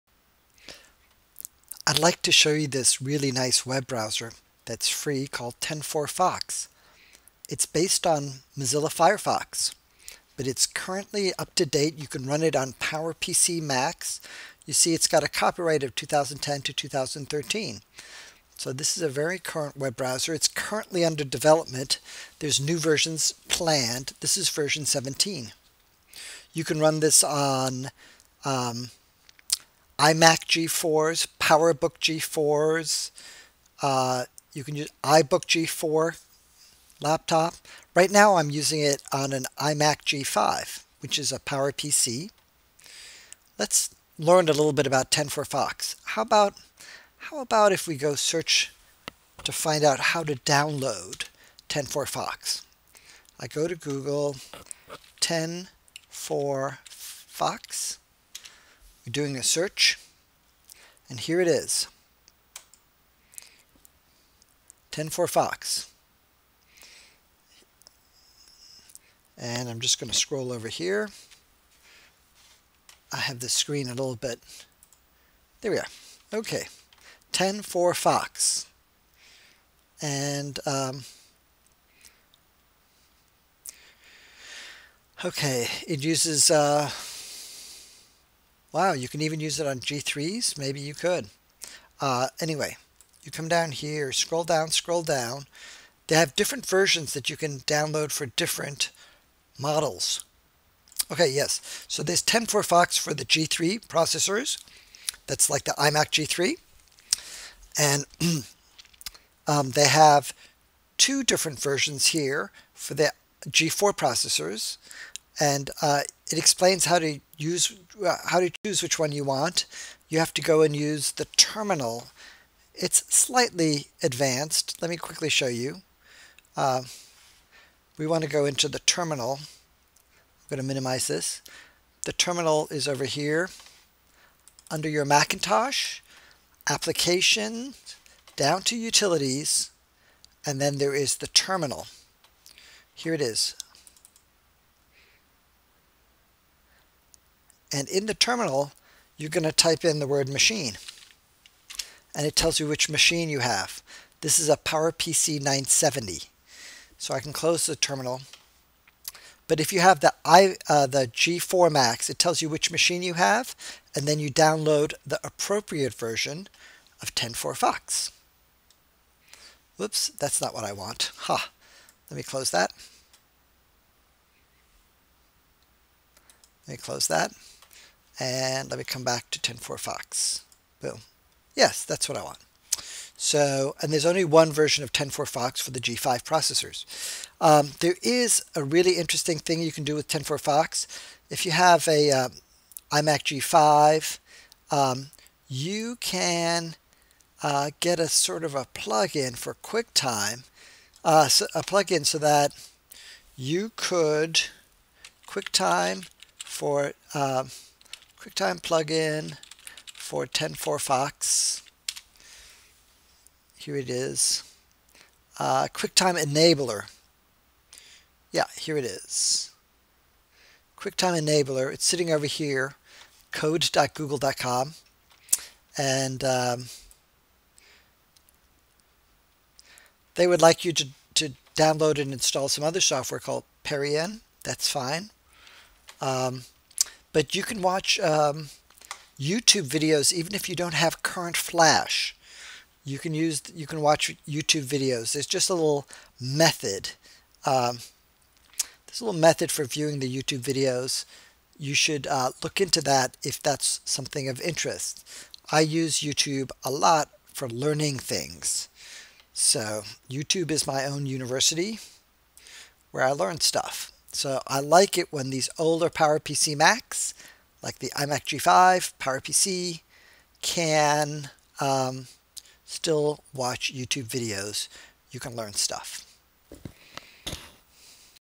Using TenFourFox browser on PowerPC Macs
This wonderful browser runs on iMac G3's, PowerBook G3, iBook G3's, iMac G4's, PowerBook G4's, iBook G4's, and iMac G5's.  All you need is Mac OS 10.4 (Tiger) or higher.  This browser is based on Mozilla Firefox -- but is totally up-to-date and current with web technologies.  (Mozilla Firefox stopped development for PowerPC Macs about two years ago.)